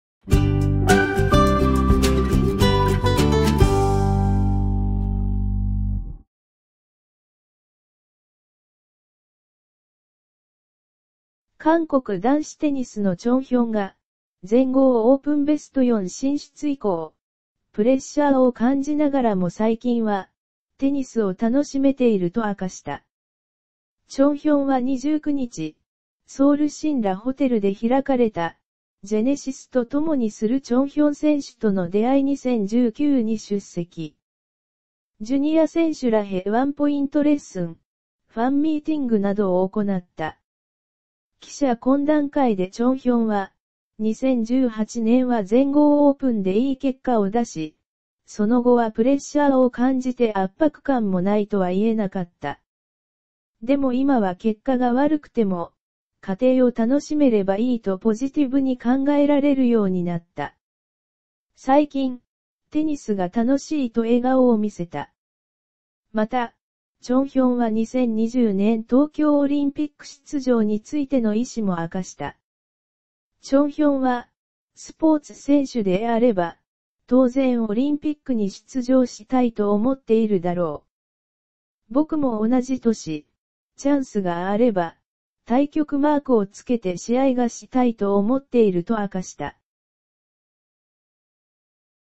韓国男子テニスのチョン・ヒョン、東京五輪に意気込み 「全豪4強はプレッシャーだが、五輪目指したい」 (11/29)
韓国男子テニスのチョン・ヒョン、東京五輪に意気込み 「全豪4強はプレッシャーだが、五輪目指したい」

韓国男子テニスのチョン・ヒョン（23）が、全豪オープンベスト4進出以降、プレッシャーを感じながらも最近は「テニスを楽しめている」と明かした。

　チョン・ヒョンは29日、ソウル新羅ホテルで開かれた「GENESISと共にするチョン・ヒョン選手との出会い2019」に出席。ジュニア選手らへワンポイントレッスン、ファンミーティングなどをおこなった。

　記者懇談会でチョン・ヒョンは「2018年は全豪オープンで良い結果を出し、その後はプレッシャーを感じて圧迫感もないとは言えなかった。でも今は結果が悪くても、過程を楽しめれば良いとポジティブに考えられるようになった。最近、テニスが楽しい」と笑顔をみせた。

　また、チョン・ヒョンは2020年東京オリンピック出場についての意思も明かした。チョン・ヒョンは「スポーツ選手であれば、当然オリンピックに出場したいと思っているだろう。僕も同じ」とし、「チャンスがあれば、太極マーク（国旗）を付けて試合がしたいと思っている」と明かした。